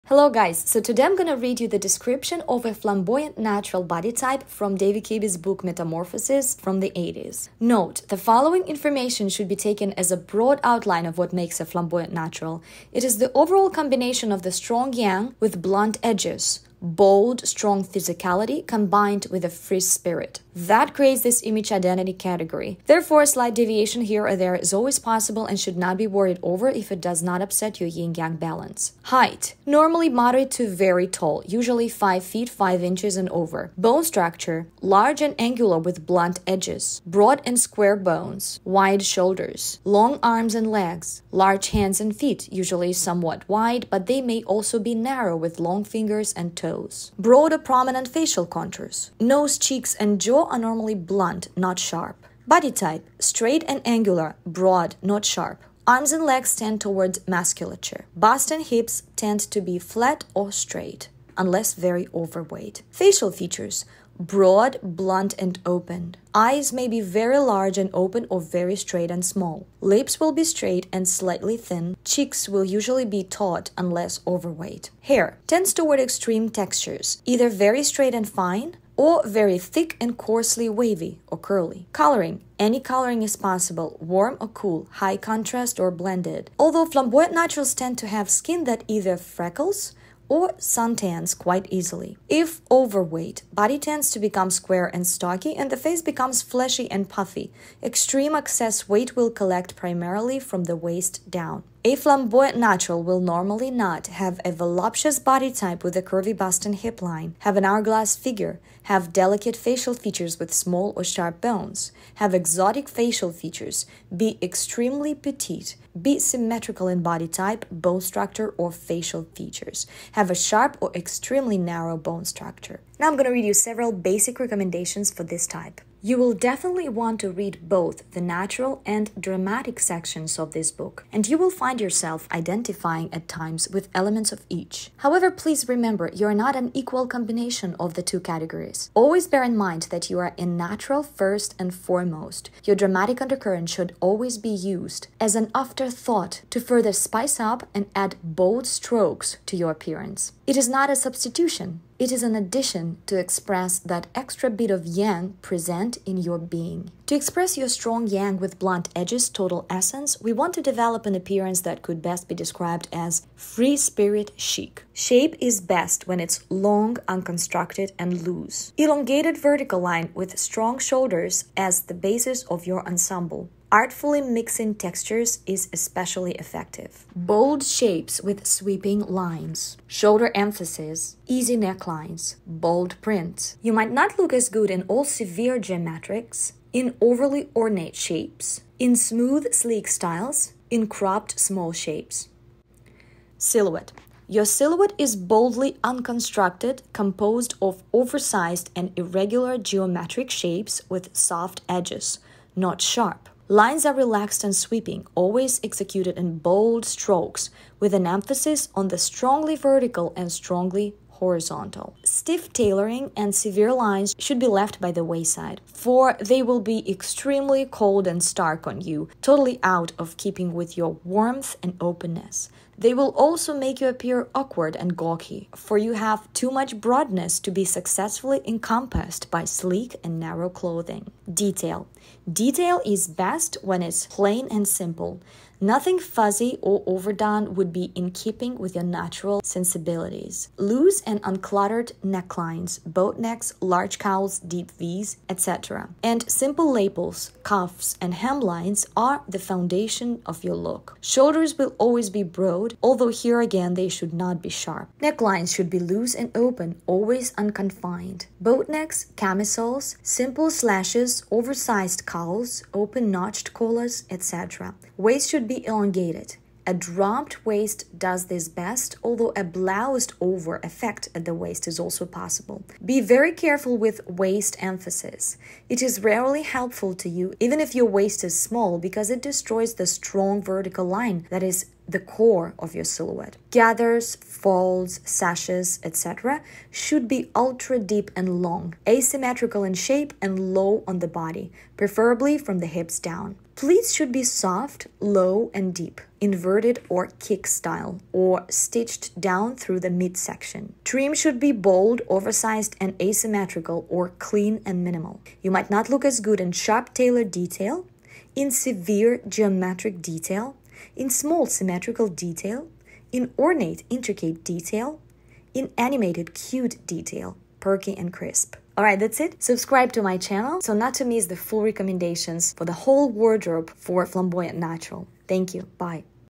YOU are FLAMBOYANT NATURAL if you have...
FLAMBOYANT NATURAL - Full Description & Basic Recommendations
Reading from the book written by David Kibbe in the 80s. So some recommendations can sound a bit old fashioned to you.

🎁  If you want to support me and get some exclusive stuff:
If you click to JOIN me you will get early access to some of my videos and soon I will add some exclusives there too!